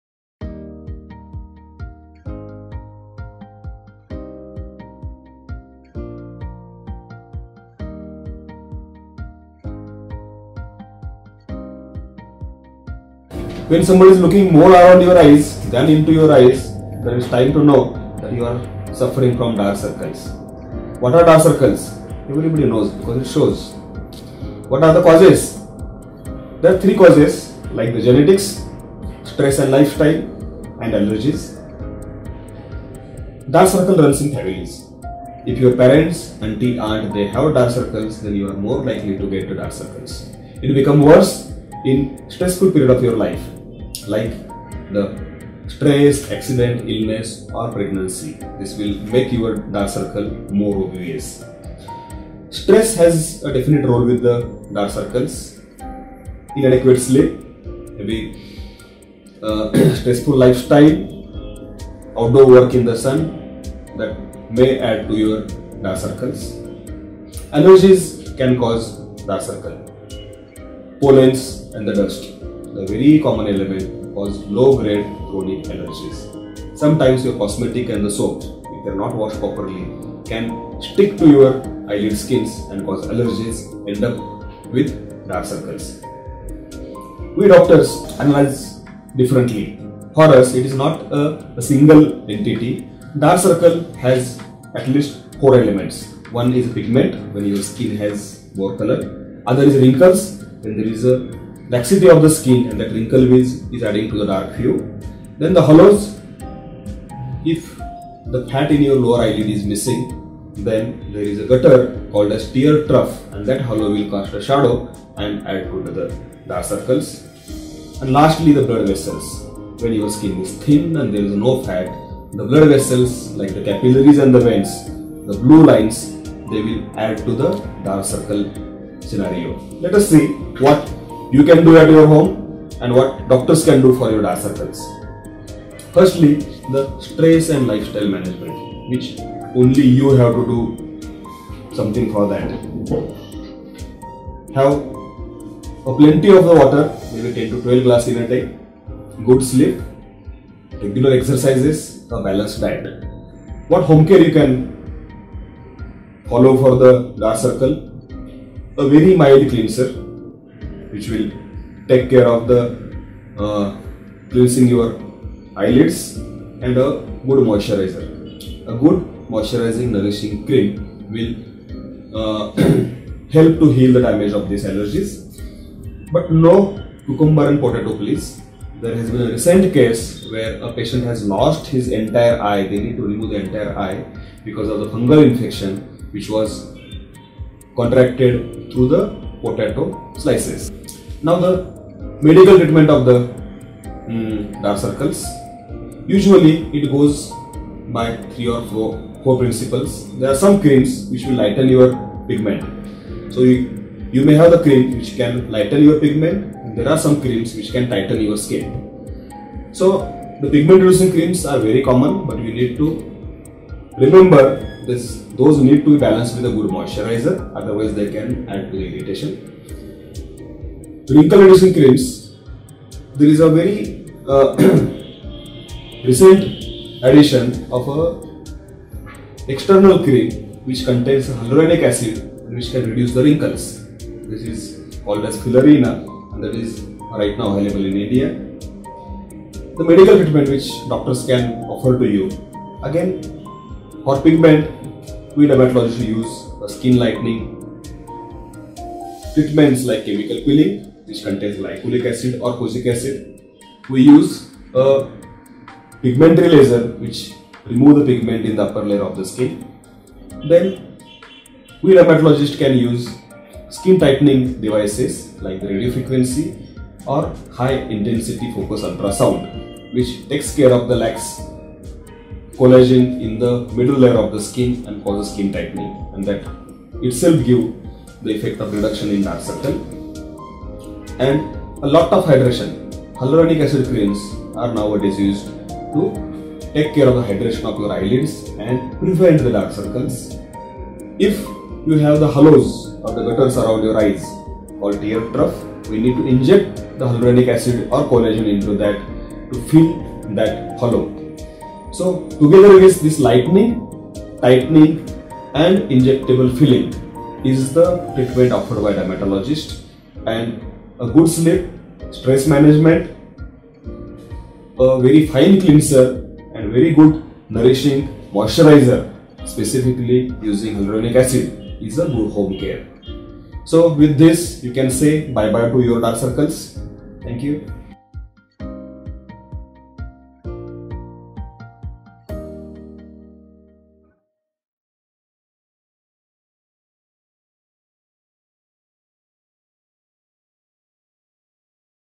Dark Circle & It’s Causes | Dark Circle Removal Treatment - Dr. Dhepe
Are you tired of looking tired? Wonder why it occurs?                                              Watch Dr Niteen Dhepe tell you all about dark circles, its causes, and its latest treatment.
At Skin City India, we can cure.
Starting from dark circles treatment, under eyes to dark spots removal, we have everything that you need. With us, put all your worries to rest.